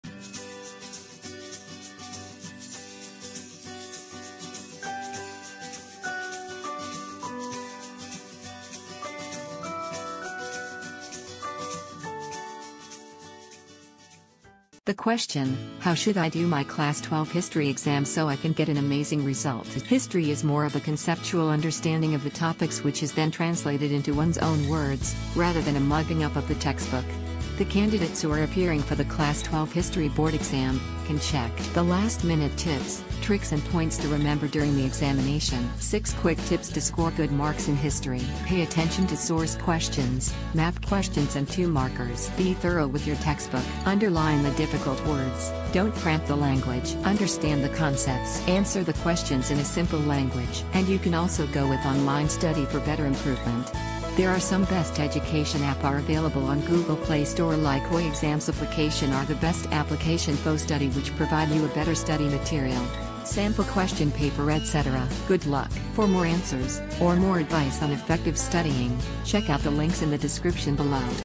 How Should I Do My Class 12 History Exam So I Can Get An Amazing Result?
There is a reason 'work smart not hard' is a cliche. Your focus should always be on being efficient with your study time. Check out the StudyTV Memory Hack on getting twice the results in half the time

History is more of a conceptual understanding of the topics which is then translated into one's own words, rather than a mugging up of the textbook. The candidates who are appearing for the class 12 History board exam, can check

Join us on live daily study sessions on our Facebook